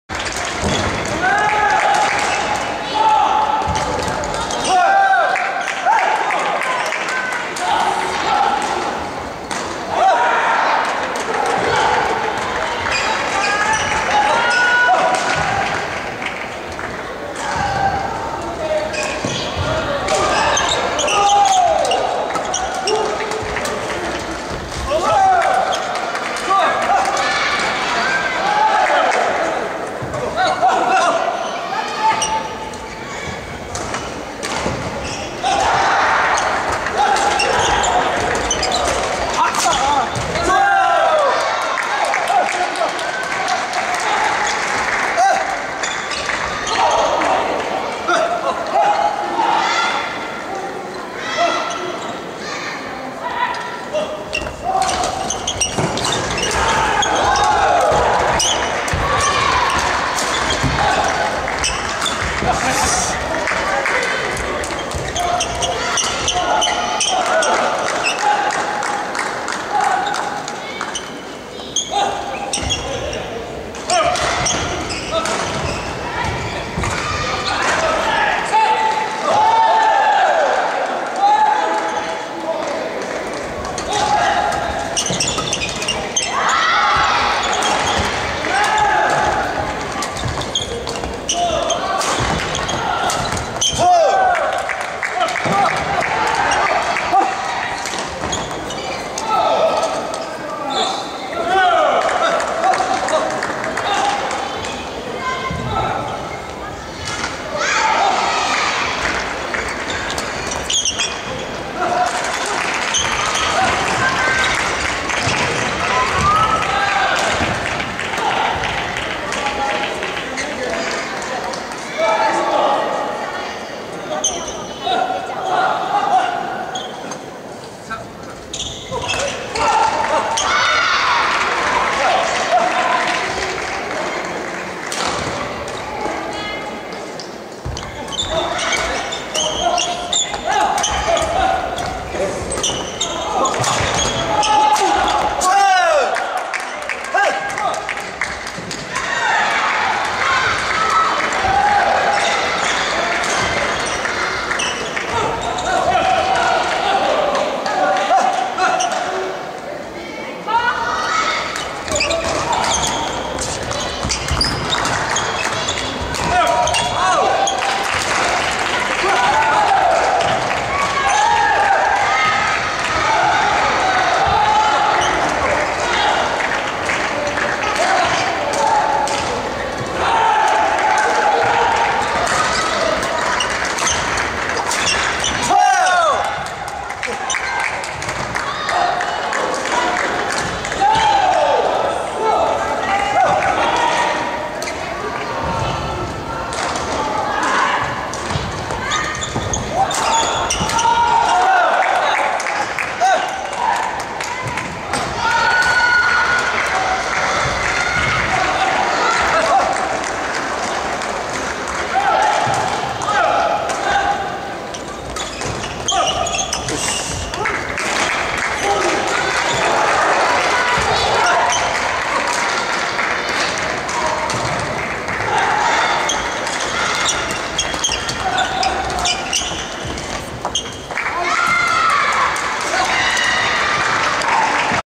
星野翔平・竹内義憲VS小林徹太郎・内藤浩司（男子ダブルス1回戦）
日本ランキングサーキット2013
6月1日(土)～6月5日(水)　埼玉県さいたま市記念総合体育館